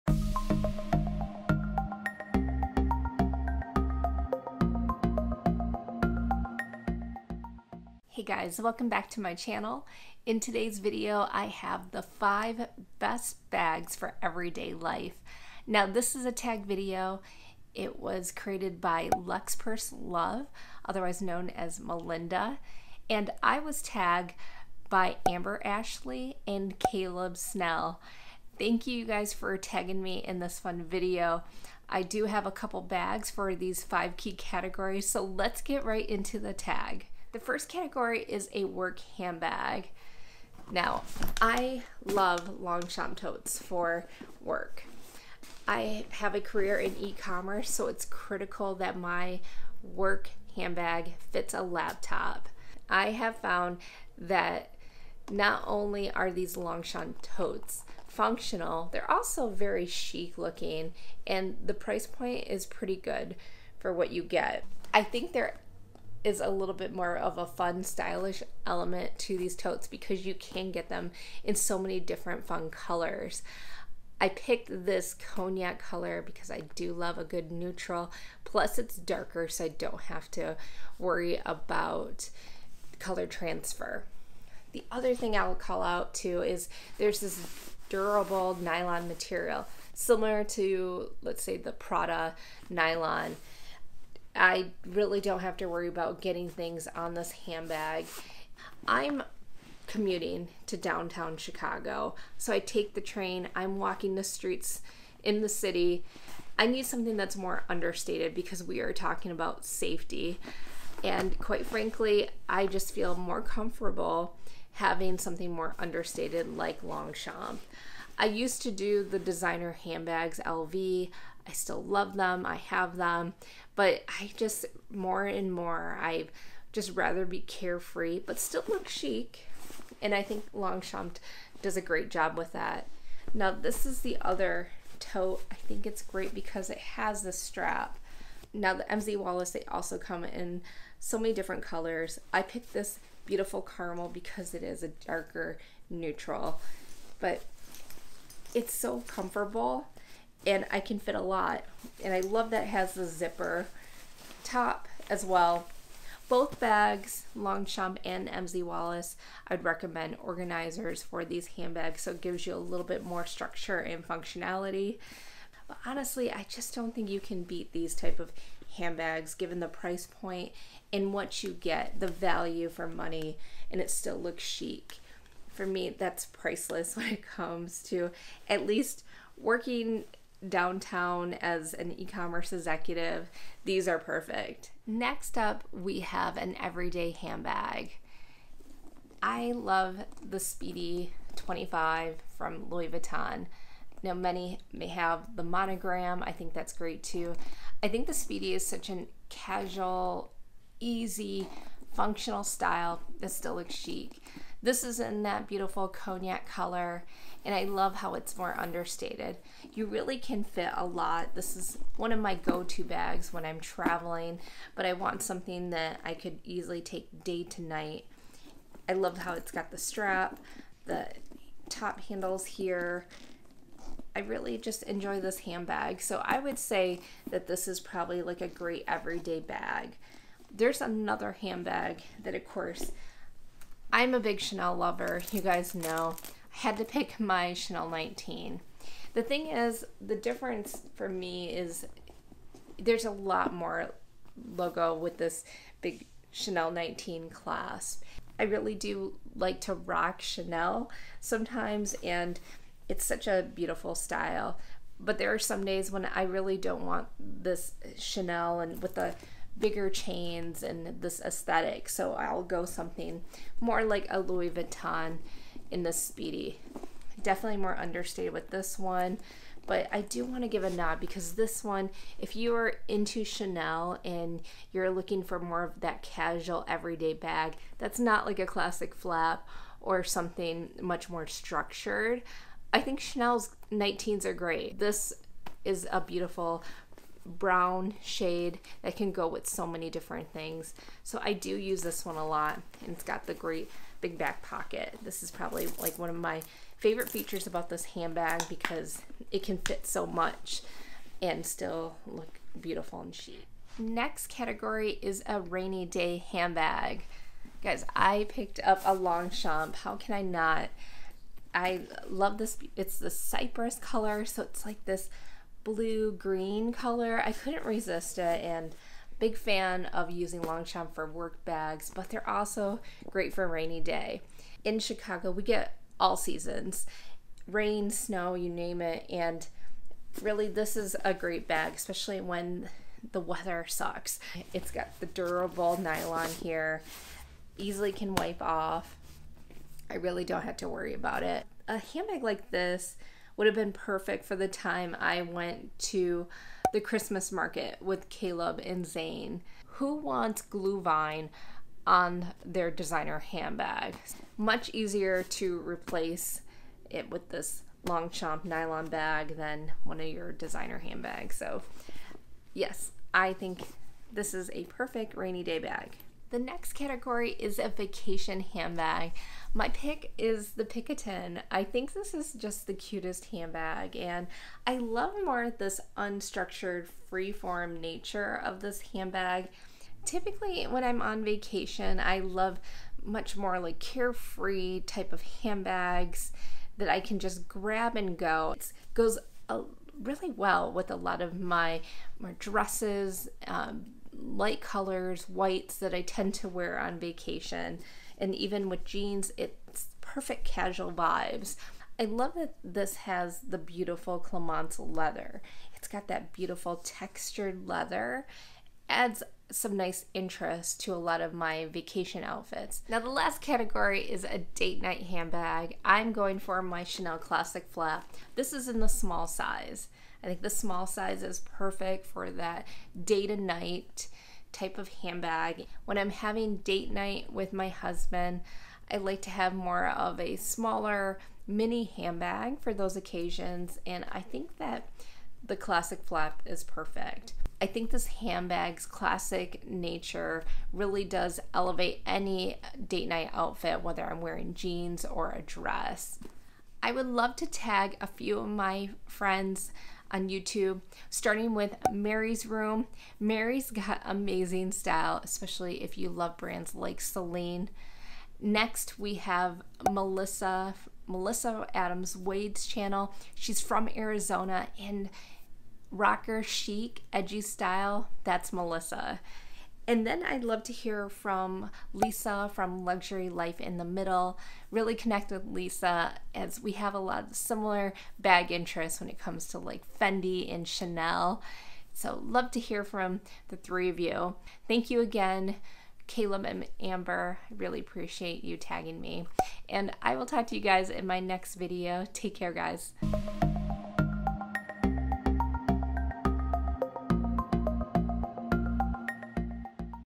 Best 5 Everyday Bags! | Tagged by @AmberAshleigh35 @CalebSnell , Created
In today's video I am featuring the 5 Best Bags for Everyday tag video.  I was tagged by @AmberAshleigh35 and @CalebSnell  and created by @LuxPurselove

Please comments below your thoughts and videos suggestions.
Jamie

Products featured:
Stripe sweater =